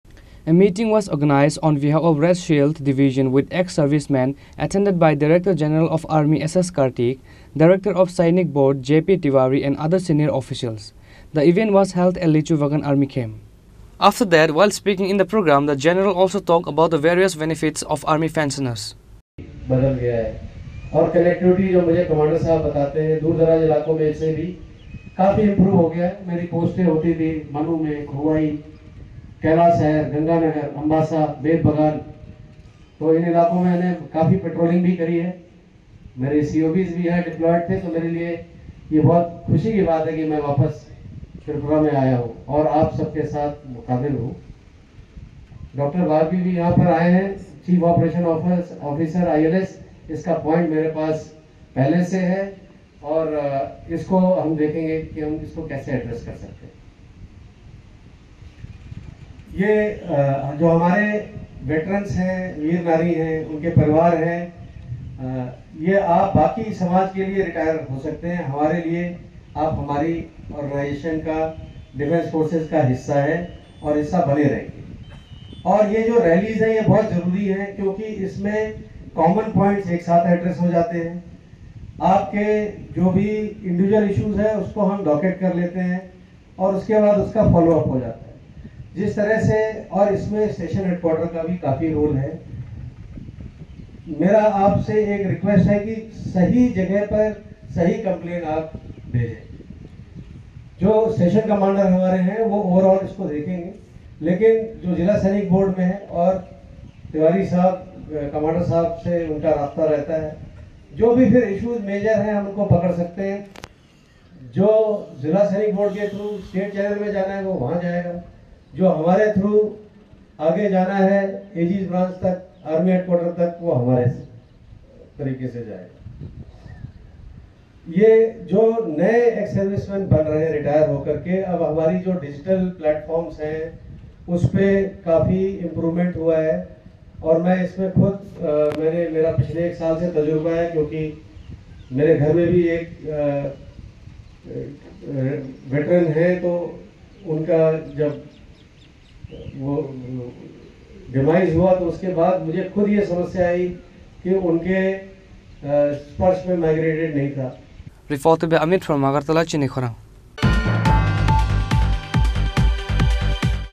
A meeting was organized on behalf of Red Shield Division with EX Servicemen
🟢A meeting was organized on behalf of Red Shield Division with EX Servicemen attended by Director General of Army SS Kartik, Director of Sainik Board JP Tibari and other senior officials. The event was held at Lichubagan Army Camp.